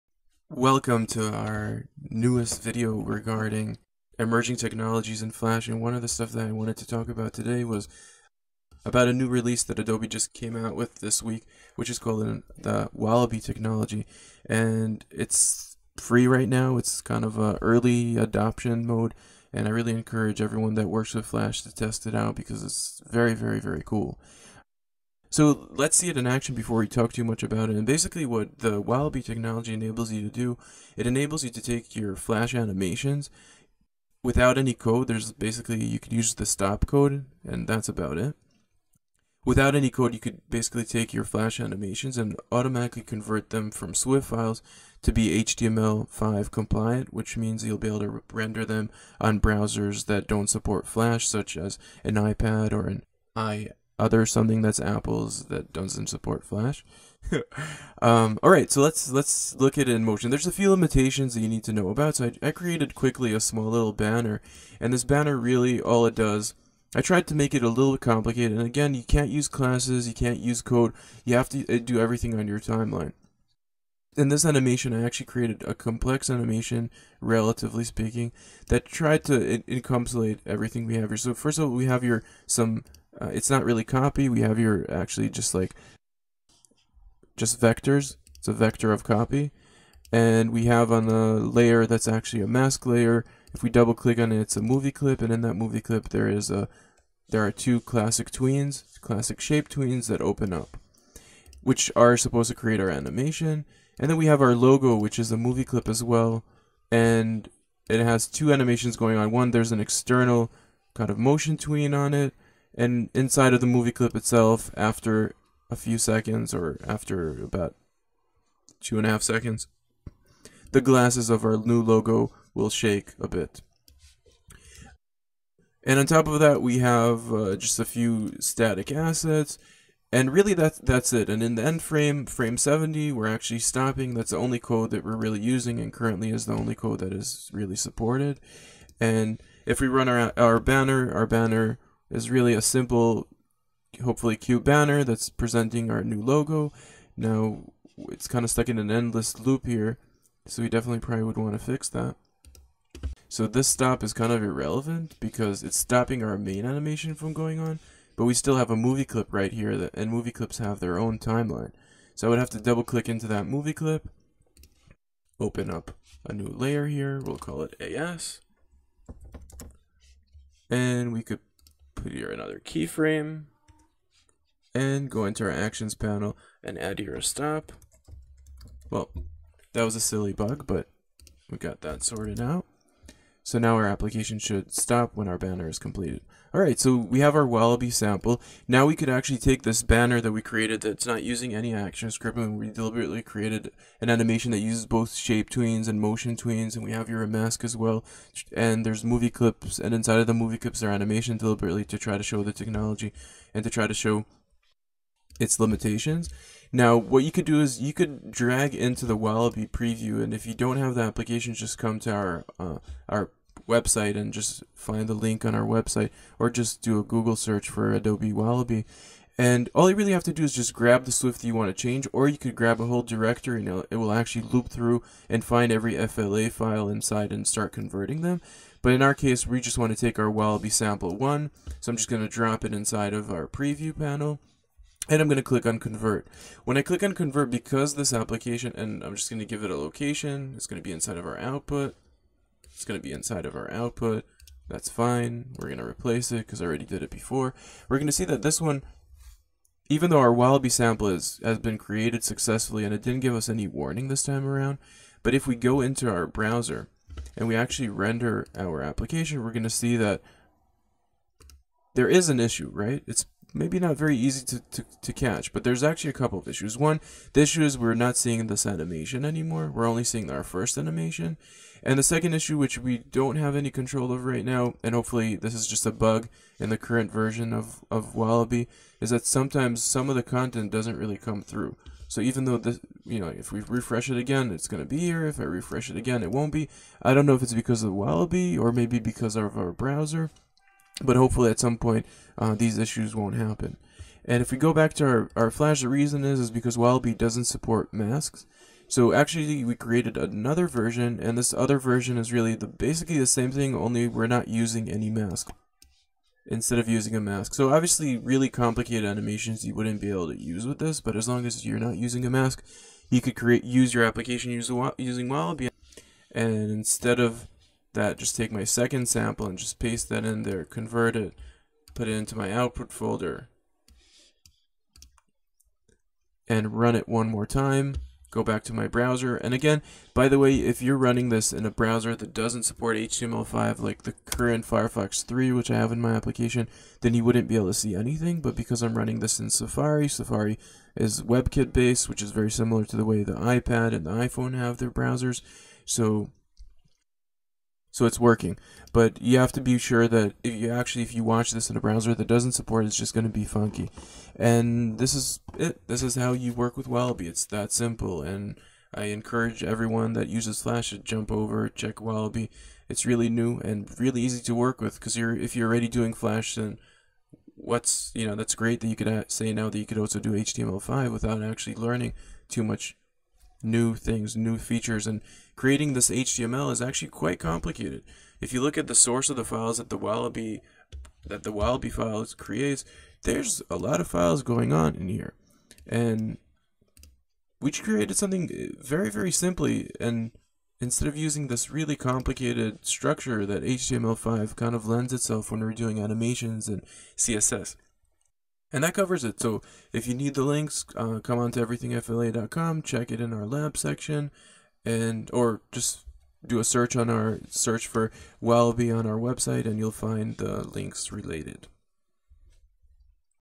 Adobe Wallaby- How to convert Flash Fla Files into HTML5
This is one of the coolest new emerging HTML5 related technologies making creating HTML5 animations so easy for us Flash Designers/Developers. In this video we will walk through the basics of it and see wha its limitations are at this current time.